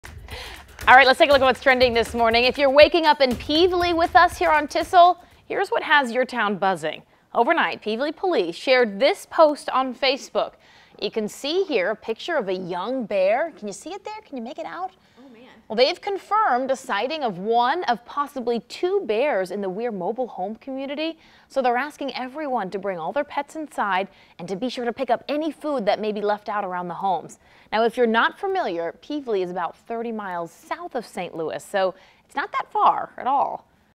Pevely Police shared this post on Facebook of a picture of a young bear
The police say they have a confirmed sighting of one of possibly two bears